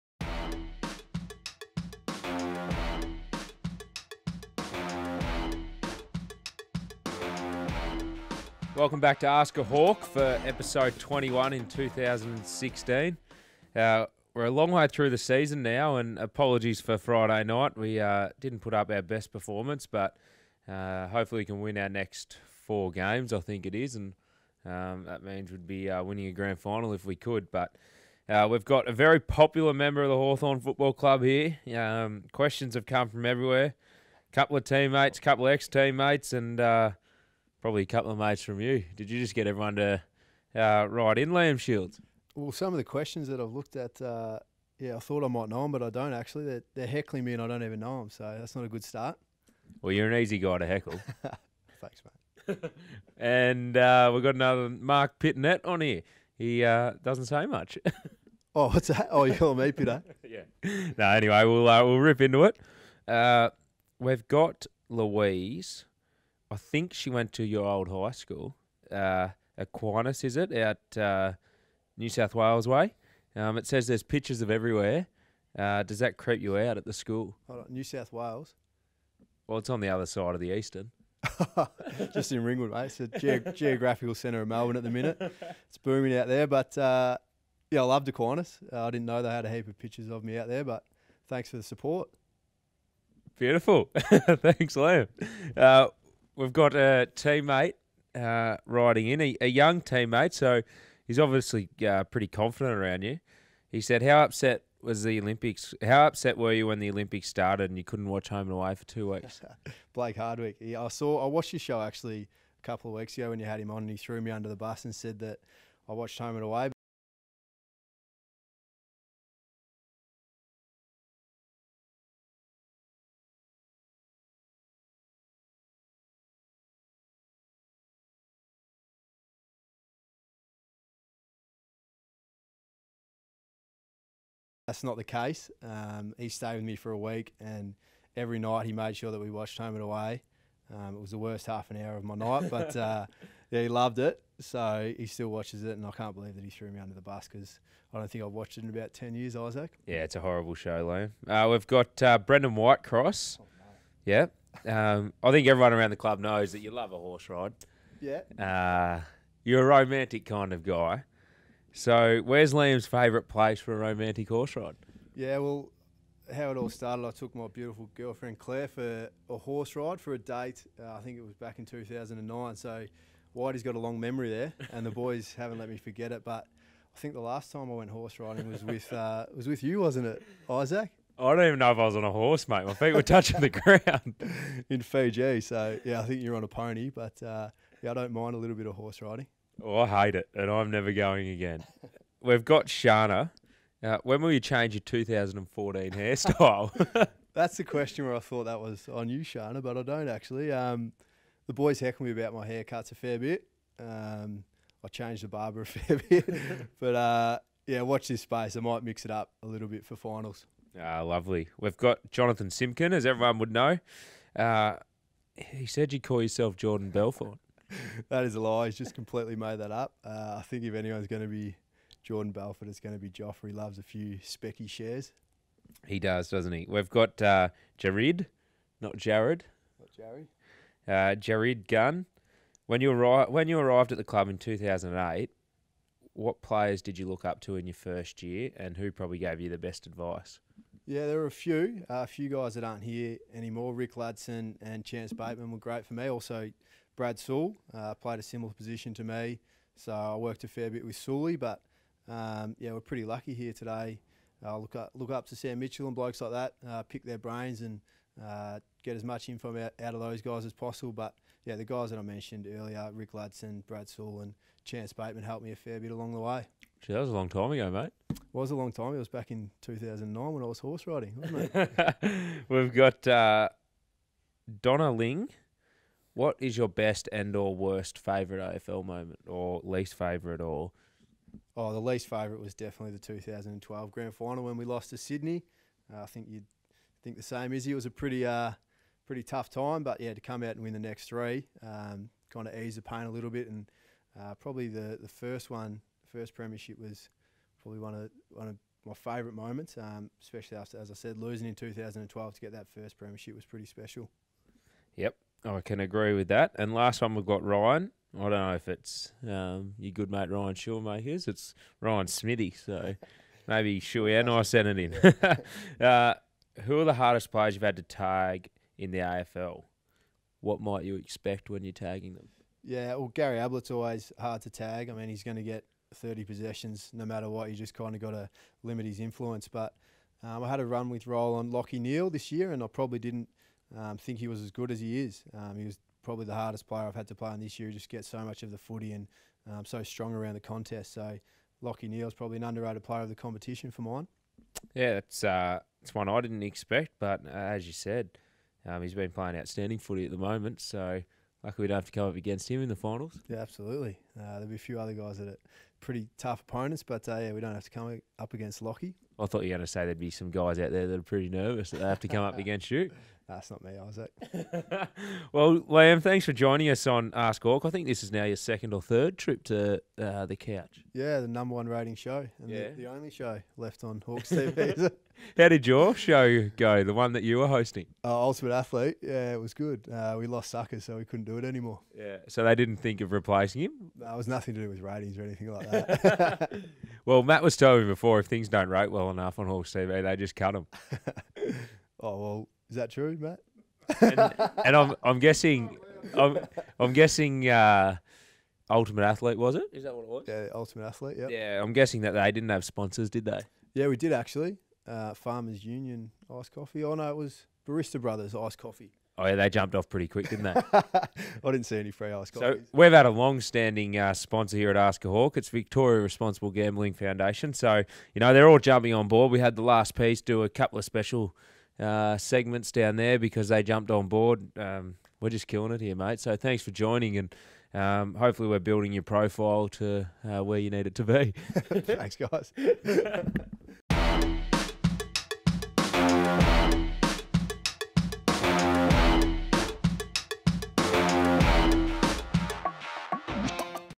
Ask a Hawk | Liam Shiels | Episode 21 2016
Ask a Hawk 2016 ep21 Liam Shiels with Isaac Smith